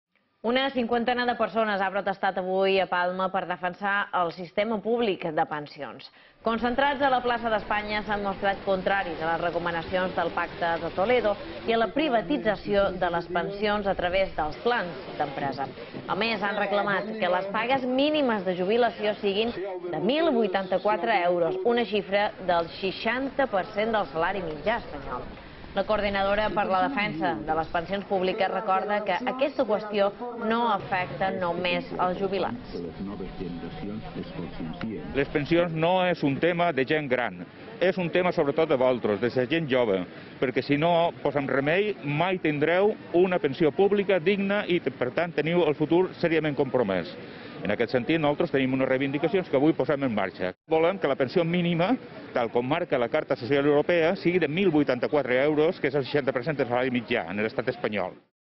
Una cinquantena de persones protesta a Palma per defensar el sistema públic de pensions
Una cinquantena de persones ha protestat avui a Palma per defensar el sistema públic de pensions. Concentrats a la plaça d’Espanya, s’han mostrat contraris a les recomanacions del Pacte de Toledo i a la privatització de les pensions a través dels plans d’empresa.

A més, han reclamat que les pagues mínimes de jubilació siguin de 1.084 euros, una xifra del 60 per cent del salari mitjà espanyol. La Coordinadora per la Defensa de les Pensions Públiques recorda que aquesta qüestió no afecta només els jubilats, sinó sobretot els joves.

Pep Juárez, portaveu de la Coordinadora: “les pensions no és un tema de gent gran, és un tema de la gent jove, perquè si no hi posam remei, mai tendran una pensió pública digna”.